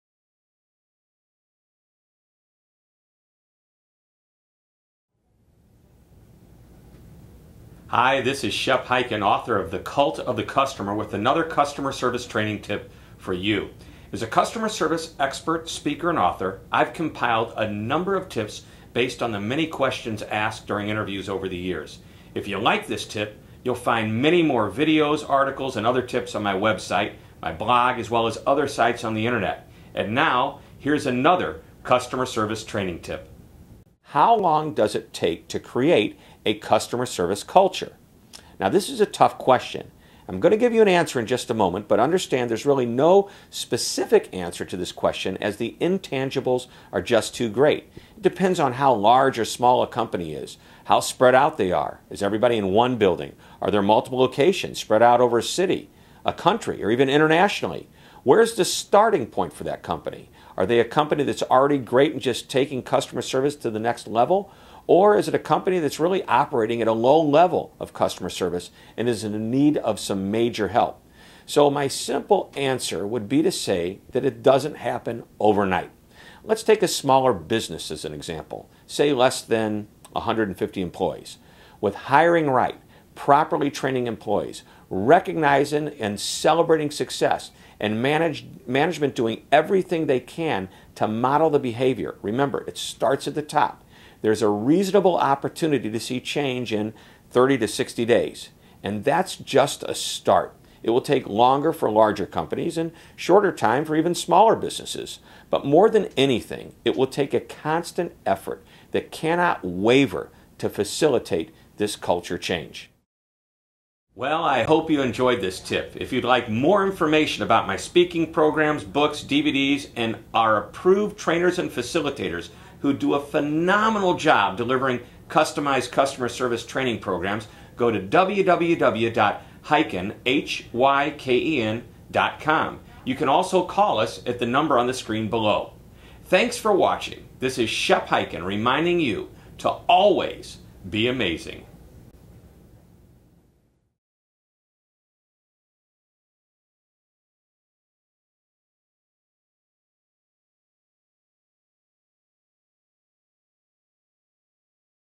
Customer Service Tip - Customer Service Culture Creation by Shep Hyken
How long does it take to create a customer service culture? Customer service speaker, author and training expert, Shep Hyken explains with a quick 2 minute video tip. For more information about The Customer Focus (the customer service training program) go